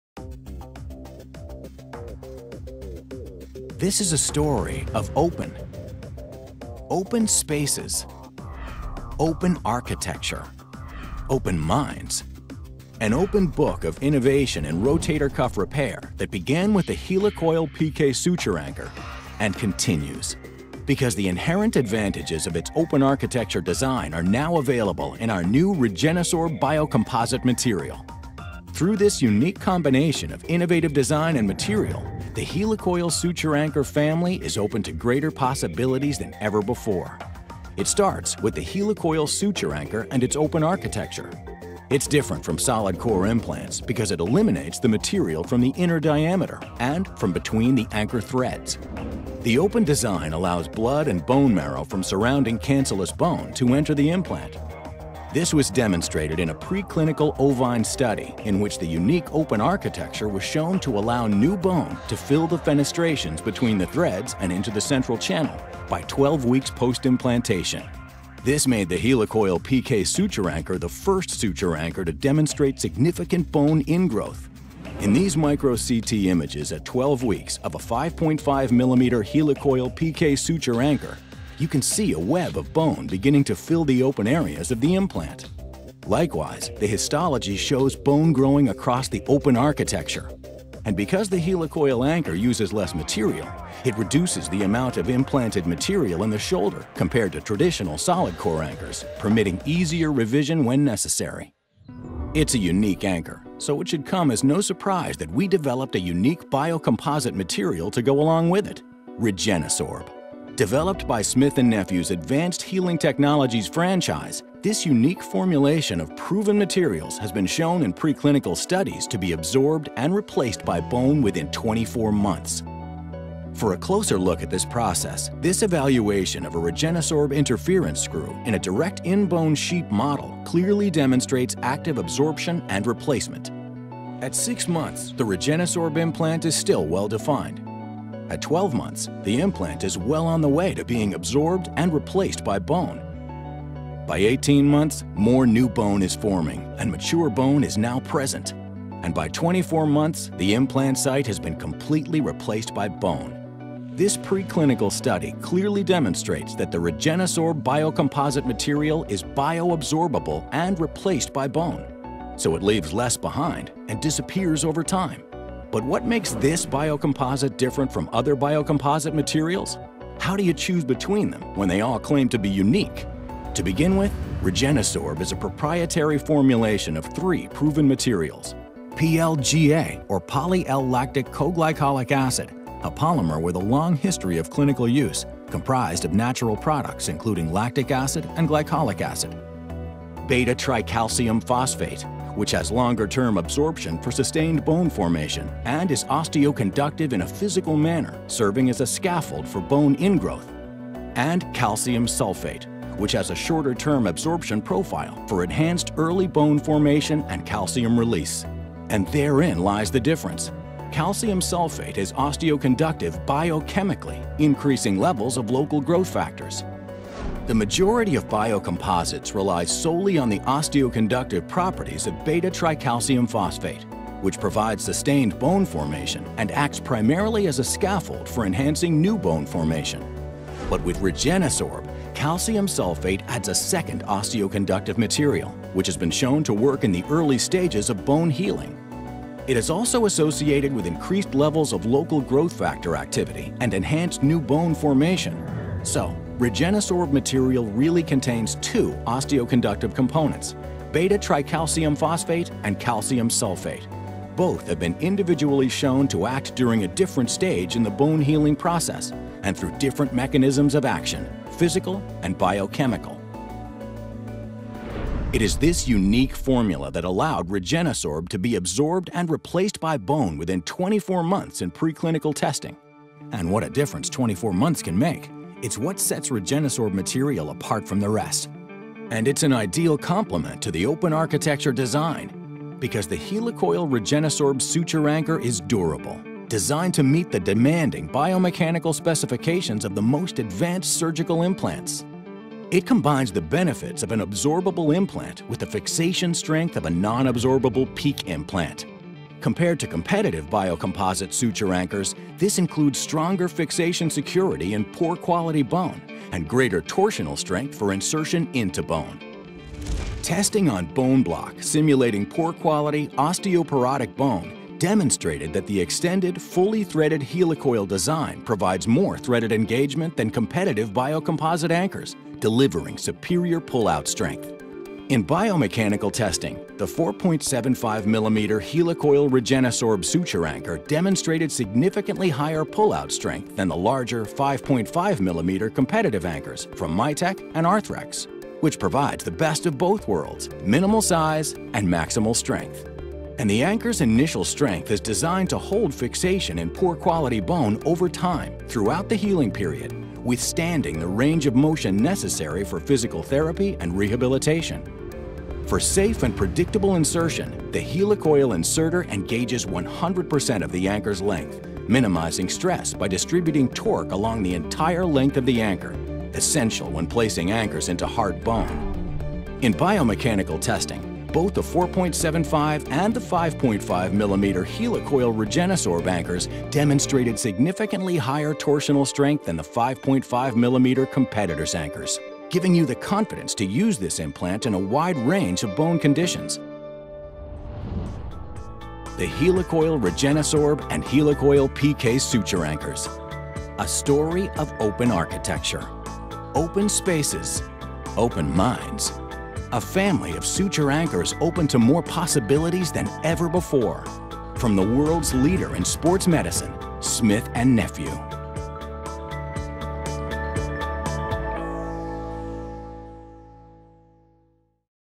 HEALICOIL* REGENESORB Suture Anchor
Building on the success of the innovative HEALICOIL PK design, this new suture anchor is the first device to use Smith & Nephew's proprietary REGENESORB material -- an advanced biocomposite that has been shown in pre-clinical studies to be absorbed and replaced by bone within 24 months.

This video is intended for health care professionals practicing in U.S. only.  It is offered for informational and educational purposes only and is not intended as medical advice.